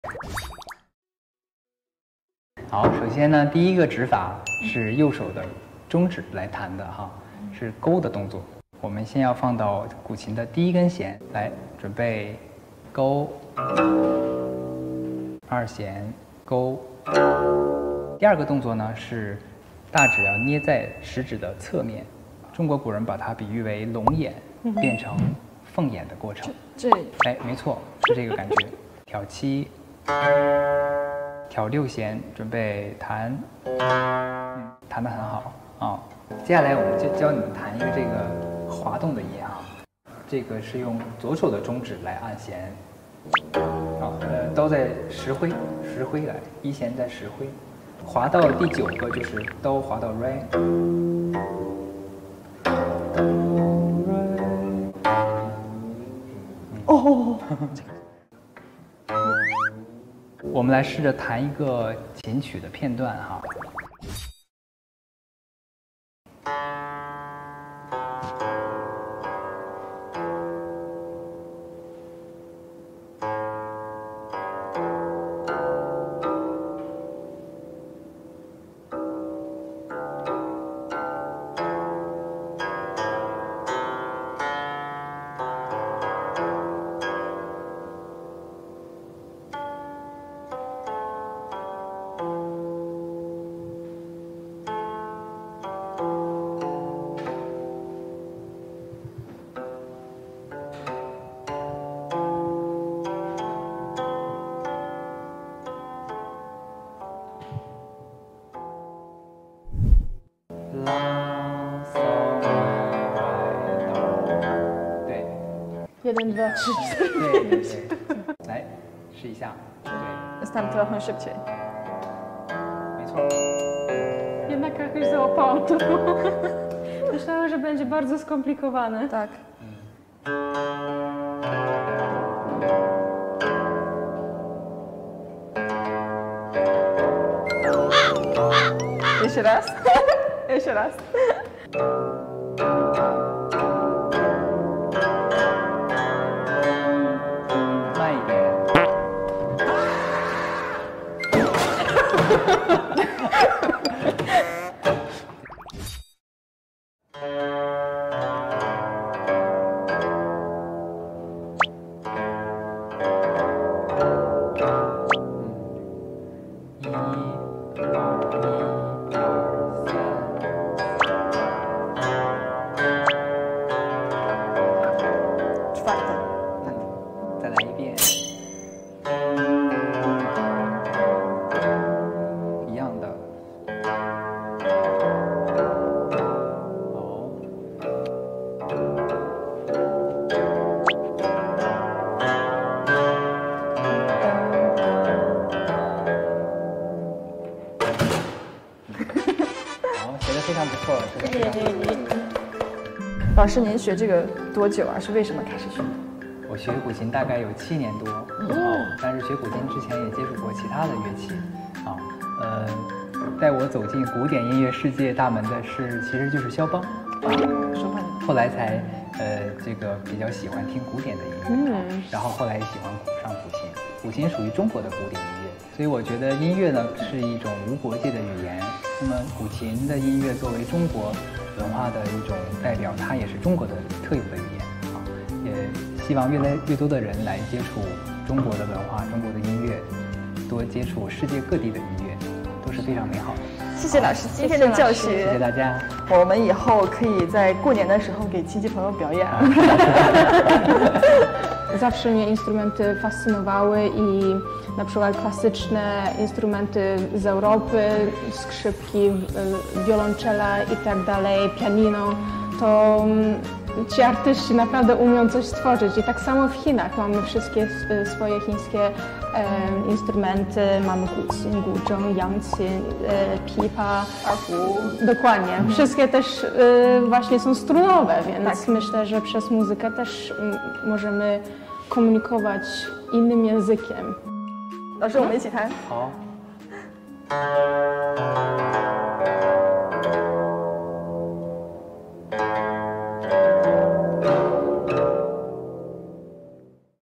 Pełna kultura, odc. 17 - Lekcja gry na guqin
Pełna kultura powraca po świątecznej przerwie. Dziś tematem przewodnim będzie muzyka. Razem z Oliwią i Wiktorią nauczymy się gry na guqin – jednym z tradycyjnych instrumentów Państwa Środka.